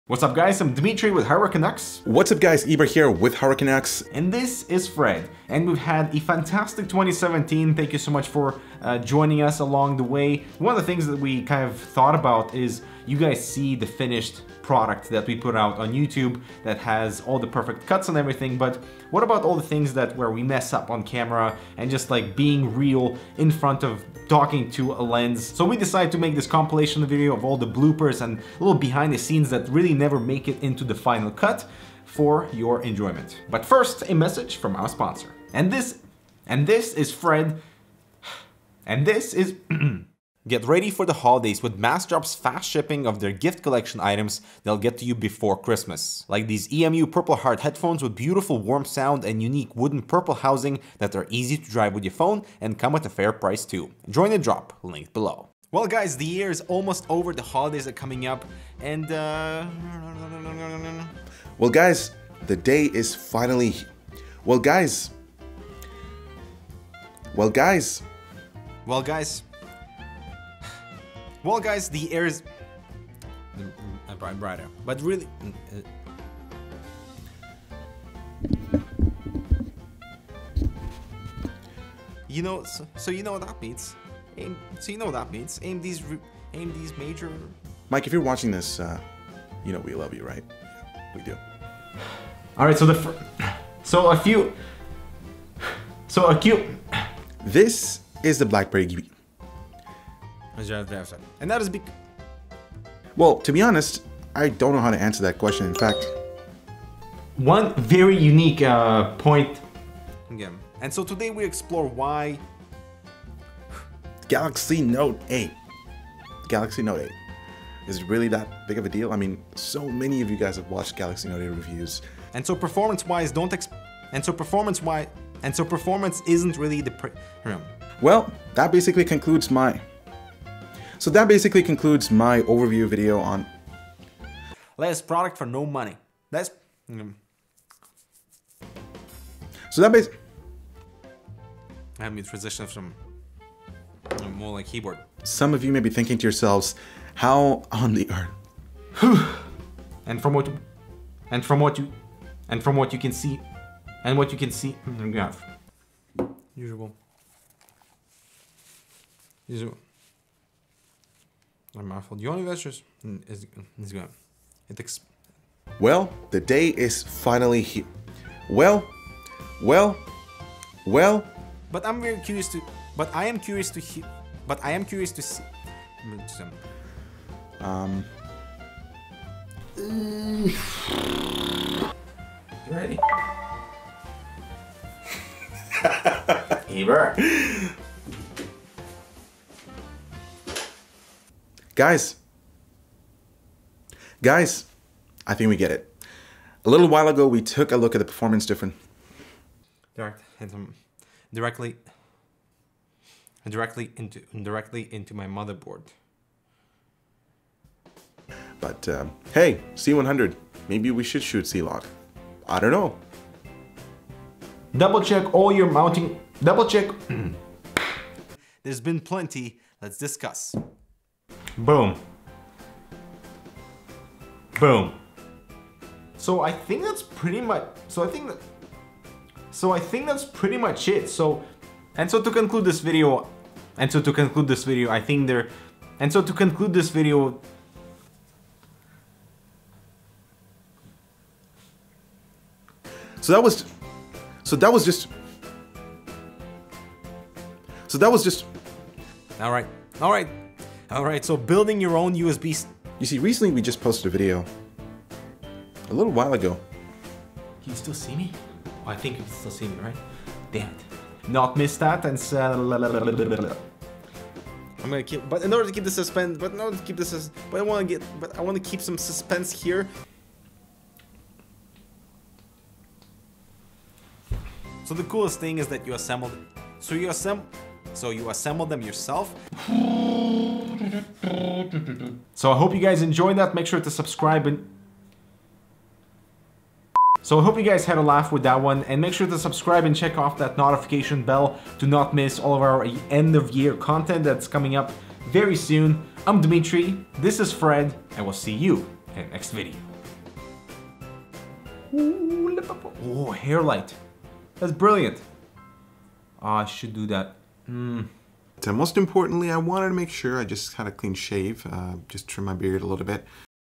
We Screw Up - The 2017 Blooper Reel
This year had seen a lot of mess ups from Eber and Dmitry on-camera so we compiled this blooper reel for you to enjoy :)